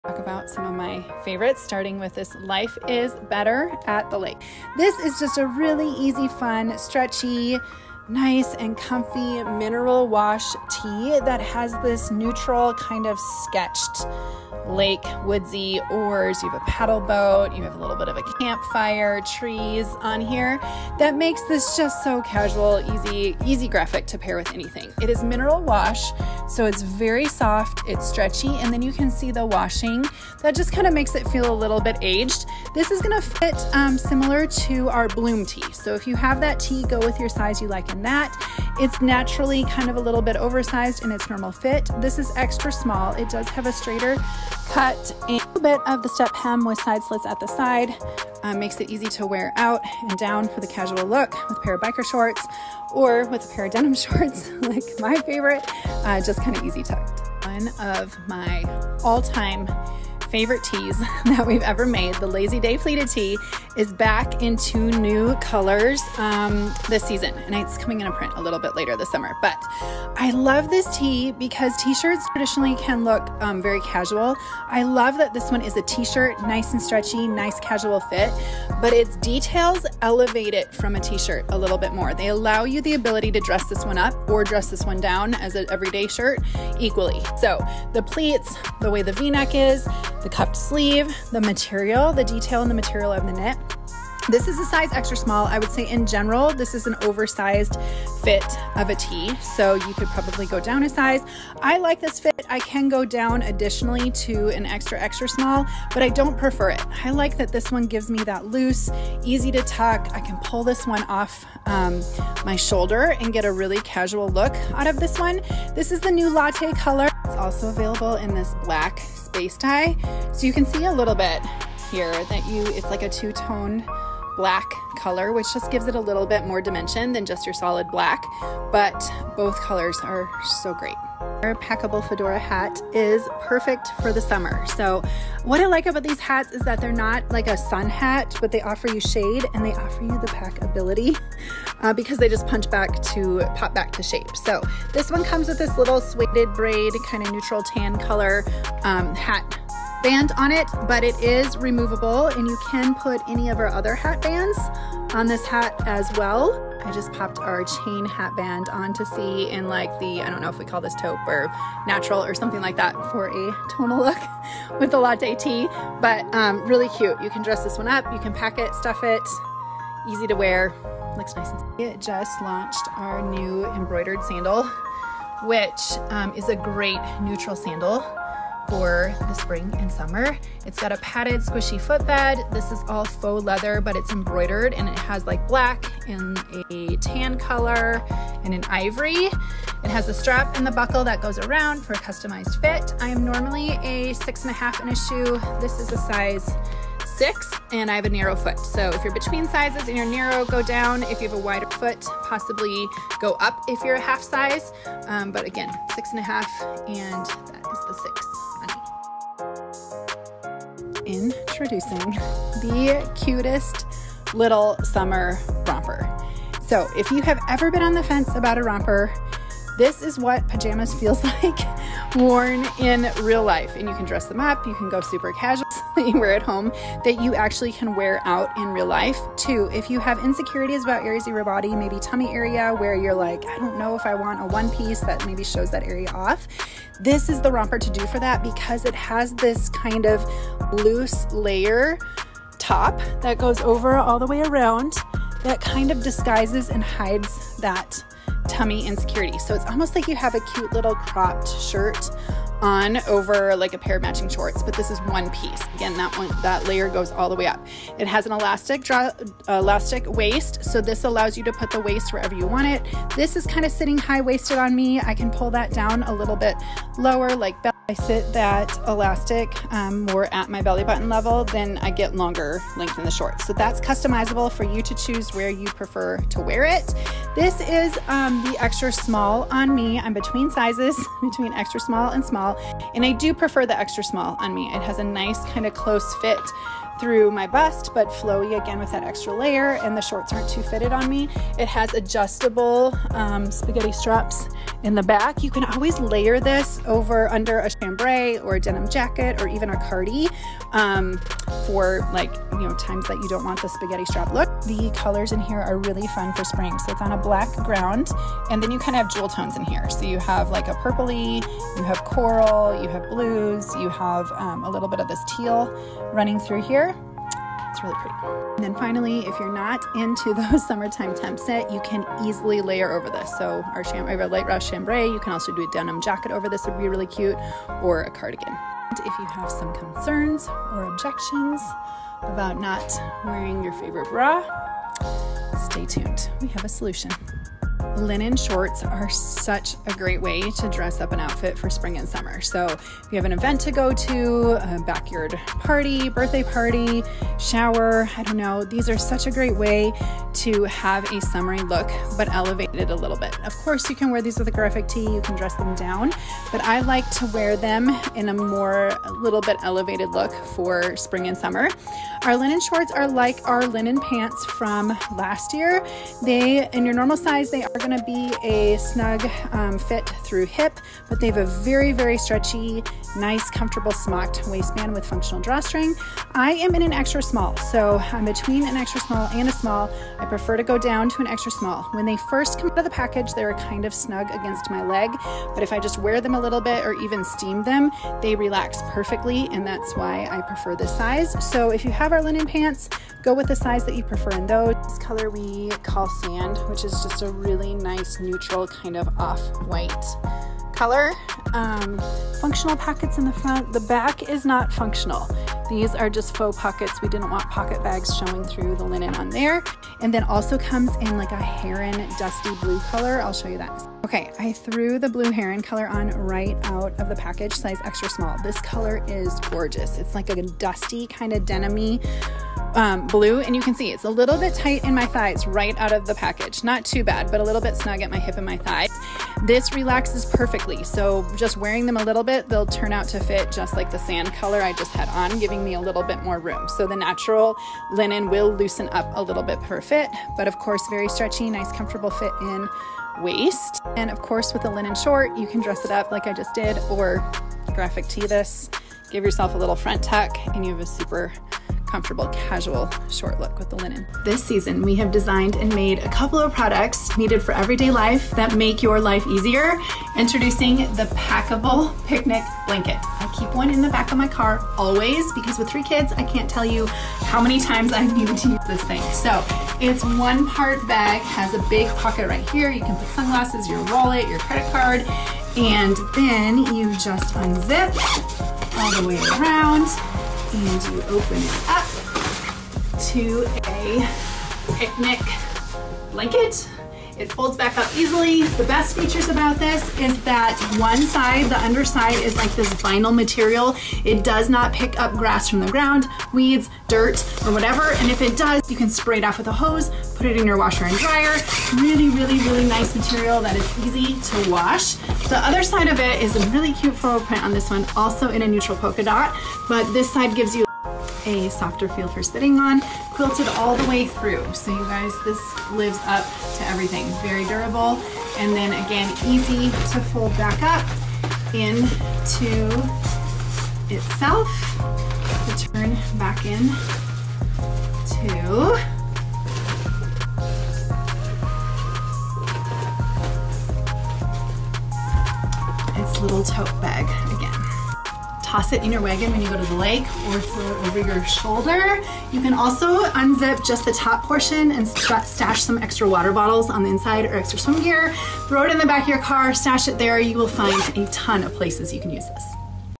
Try on with Melissa! The PERFECT Vacation Styles Are Here!
Want to get away? It is almost time for summer and we have the perfect vacation styles for you! Join Melissa in her try on sessions as she shows off the latest styles from Grace and Lace!